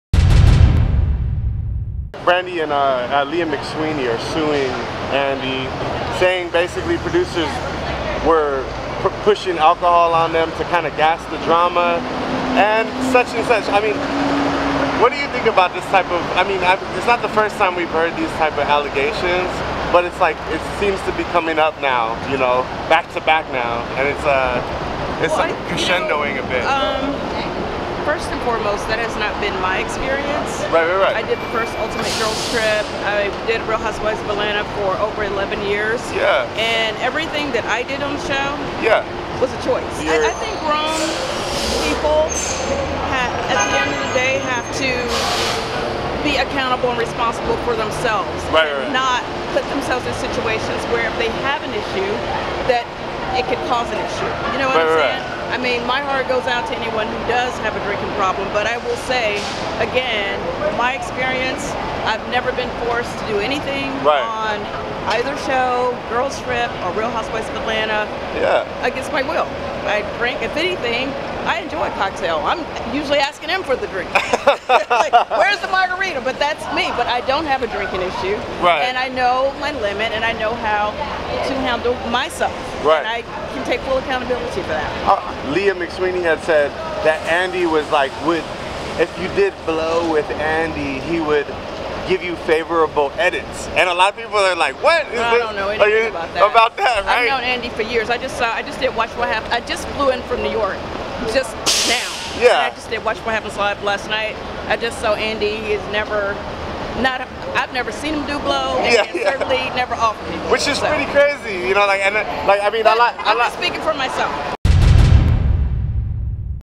Cynthia Bailey Defends Andy Cohen Amidst Leah McSweeney's Allegations | TMZ
Multiple stars of the 'Real Housewives' franchise are coming forward to defend Andy Cohen against Leah McSweeney's allegations -- and they're all leaning into the idea each cast member is personally responsible for her actions.